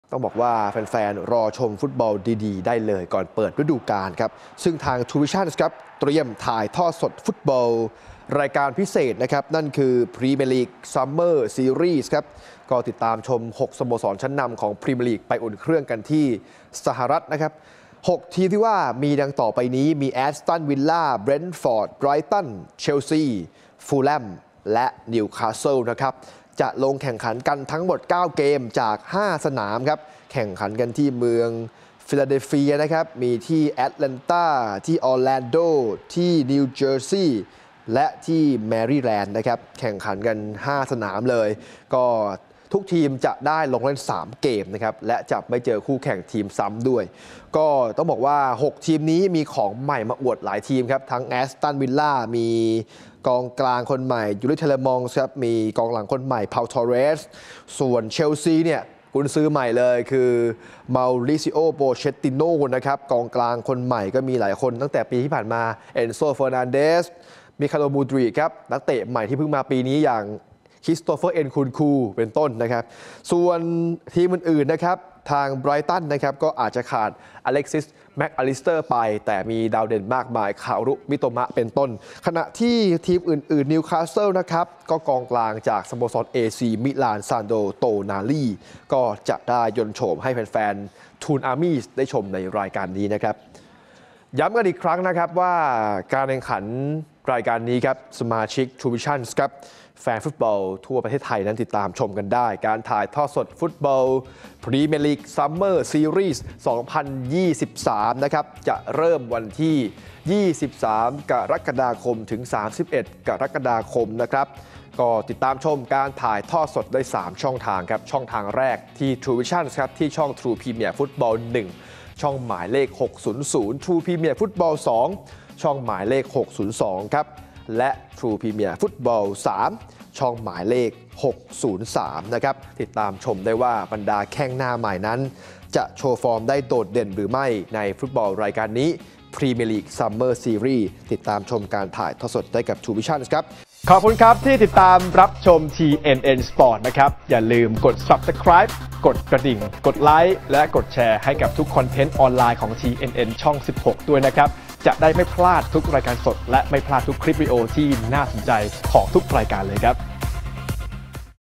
“ทรูวิชันส์” สดพรีเมียร์ลีก ซัมเมอร์ ซีรีส์ l TNNสปอร์ต
ทรูวิชั่นส์ เตรียมถ่ายทอดสดฟุตบอลทัวนาเมนต์พิเศษ พรีเมียร์ลีก ซัมเมอร์ ซีรีส์ 2023 ชมสด 6 ทีมดังจากเกาะอังกฤษบุกฟาดแข้งพรีซีซั่นที่ประเทศสหรัฐอเมริกา